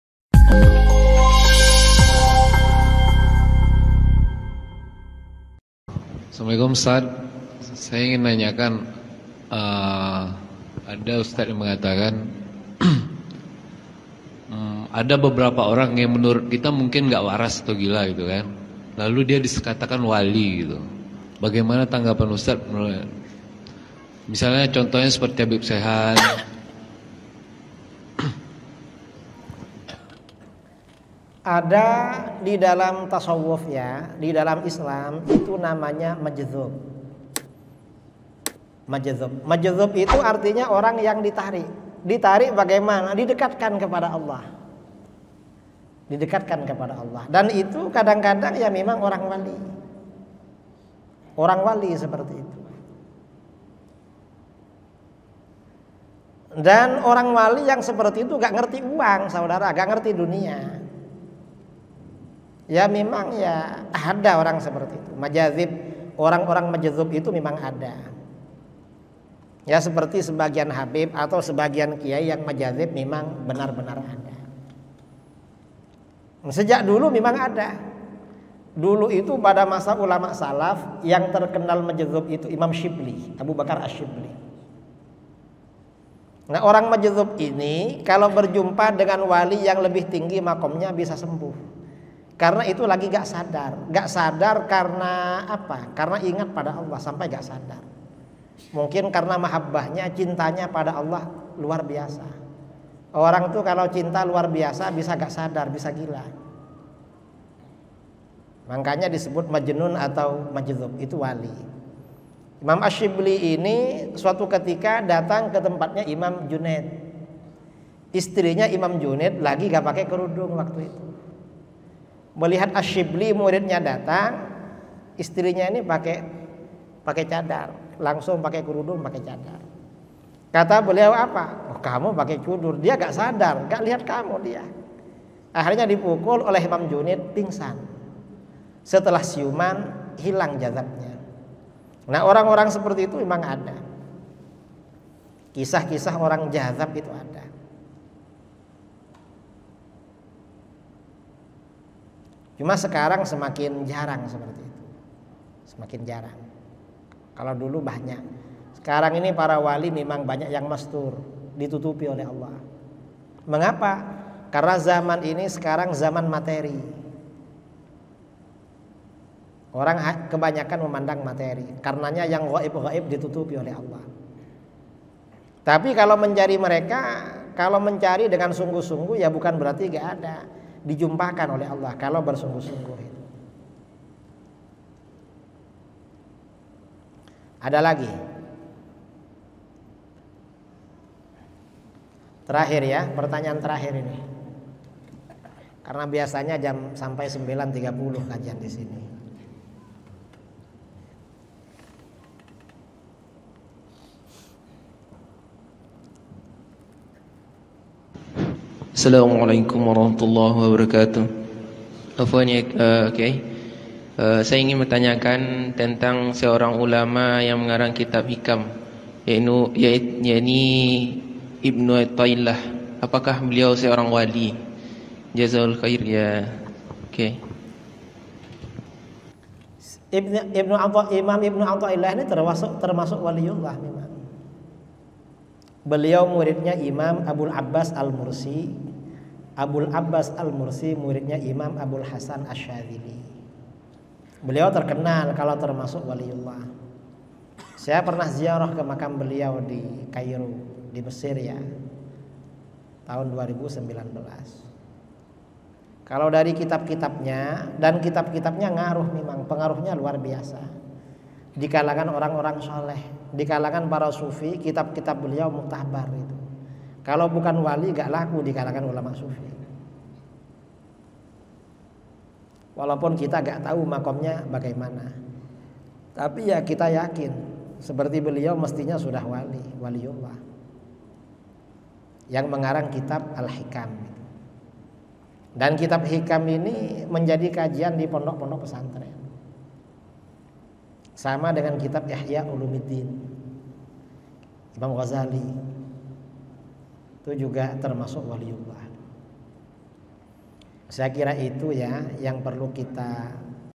Apakah orang yang kita anggap gila bisa jadi wali ? - Kiyai Idrus Ramli
Chanel Kami adalah channel Youtube yang berisikan ceramah-ceramah agama dari tuan guru ustadz DR Abdul Somad UAS, dan Asatidz Lainnya